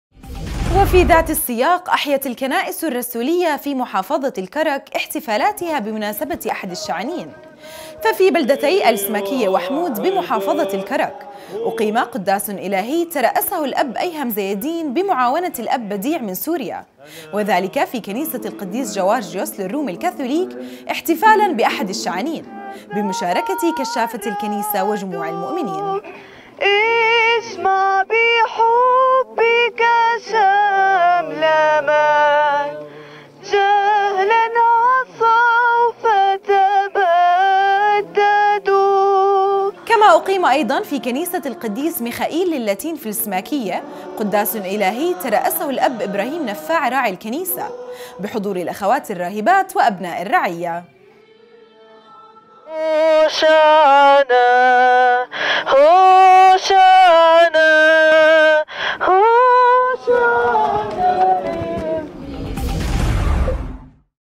نورسات الأردن : أخبار- أحيت الكنائسُ الرسولية في محافظةِ الكرك احتفالاتِها بمناسبةِ أحدِ الشعانين
وفي ذاتِ السياق أحيت الكنائسُ الرسولية في محافظةِ الكرك احتفالاتِها بمناسبةِ أحدِ الشعانين
ففي بلدتي السماكية وحمود بمحافظةِ الكرك أُقيم قداسٌ إلهي ترأسَهُ الأب ايهم زيادين بمعاونةِ الأب بديع من سوريا وذلك في كنيسةِ القديس جاورجيوس للرومِ الكاثوليك احتفالاً بأحدِ الشعانين، بمشاركةِ كشافةِ الكنيسة وجموعِ المؤمنين
2018
كما أُقيمَ ايضا في كنيسةِ القديس ميخائيل للاتين في السماكية قداسٌ إلهي ترأسَهُ الأب ابراهيم نفاع راعي الكنيسة بحضورِ الأخواتِ الراهبات وابناءِ الرعية .